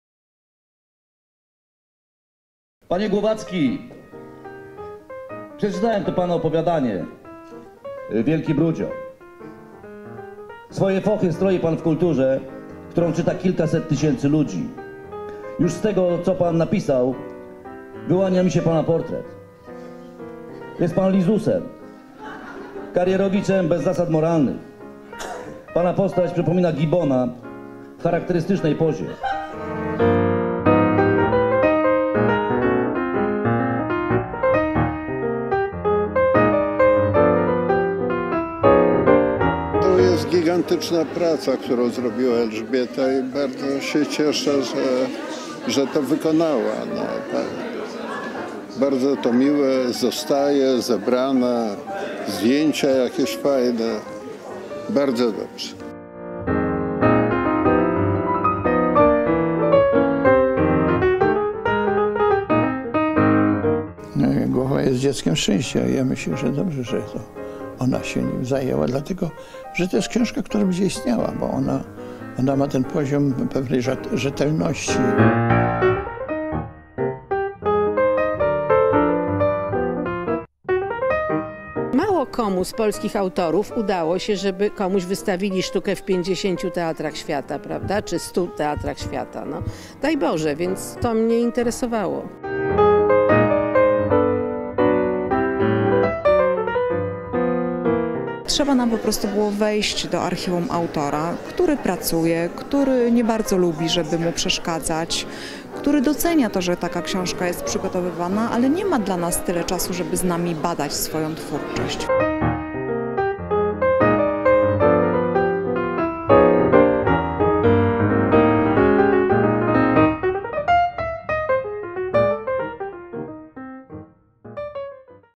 "Dżanus" w Teatrze Powszechnym w Warszawie
9 listopada 2016 roku w Teatrze Powszechnym w Warszawie odbyło się spotkanie promujące biografię Janusza Głowackiego - "Dżanus. Dramatyczne przypadki Janusza Głowackiego", napisaną przez Elżbietę Baniewicz.
To pełna zwrotów akcji opowieść o losach polskiego pisarza, który po wprowadzeniu stanu wojennego zamieszkał w Nowym Jorku.  Znany i uznany w Polsce autor opowiadań, felietonów i scenariuszy filmowych zaczynał życie za oceanem jako nikomu tam nieznany pisarz. W kilka lat stał się tam jednym z najczęściej nagradzanych dramaturgów, tłumaczonym na kilkadziesiąt języków, a jego sztuki trafiły do kilkudziesięciu teatrów w Ameryce oraz  na świecie. Grano je na scenach Waszyngtonu, Los Angeles, Nowego Yorku, Chicago, Buenos Aires, Paryża, Londynu po Tajpej, Moskwę, Bonn, Pragę, Kijów czy Madryt. Książka Elżbiety Baniewicz opowiada o tych sukcesach, ale przede wszystkim stawia  pytania: jak i dlaczego tak poważny sukces polskiego pisarza stał się możliwy. Książka świetnie udokumentowana i wspaniale zilustrowana.